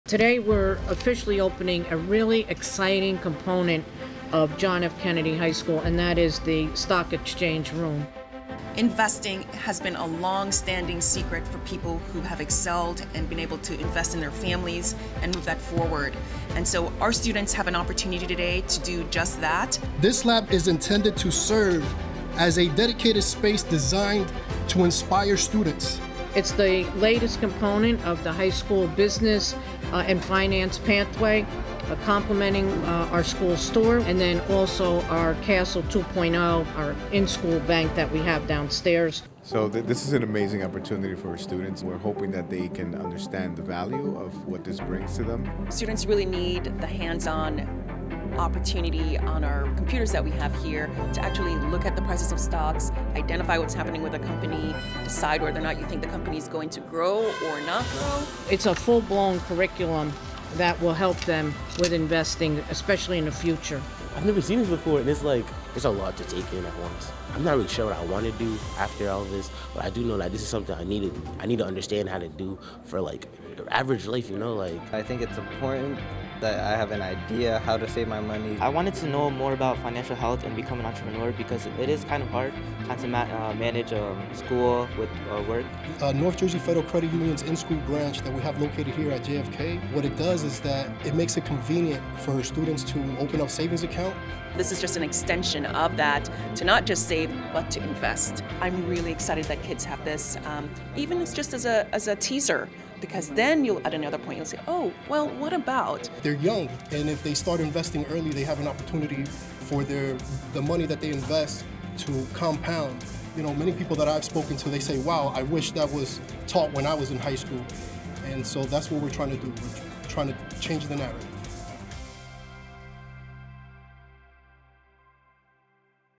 Students take stock of their fiscal futures with finance lab
Students at a New Jersey high school will soon study the stock market with a brand new room dedicated to high finance.

The finance lab was unveiled at John F. Kennedy High School in Paterson, fully equipped with new computers, a stock prices ticker and other amenities.

Students will get quasi-hands-on experience, learning what it means to be financially fit and Wall Street savvy.

"So you better have the knowledge on how to manage your money," sophomore Ludin Marroquin said. "I think this lab is cool. It's my first time ever seeing something like this."

It's a whole new environment for some eager young minds.

"We don't want them to be intimidated," a Kennedy High School faculty member said. "The same way many of our students are engaged by following a sports team, the same thing can happen with, say, a company like Tesla."

The school encourages students to put what they learn to use way before they leave high school behind.

Downstairs they have Castle 2.0, a real branch of the North Jersey Federal Credit Union where many students and teachers have accounts. Teens can earn positions as tellers.

"They take deposits, they cash checks, they open new accounts," said Martine Grant, Kennedy High School's supervisor of business. "We have students who have had summer internships with North Jersey Federal Credit Union, won scholarships for going to college."

In the finance lab, students will follow the business world in real-time.

"If the CEO of Tesla sends out a tweet, how does it impact that company," said Jorge Osoria, Kennedy's principal of Instruction.

The finance lab will be available for students to use starting in the fall semester.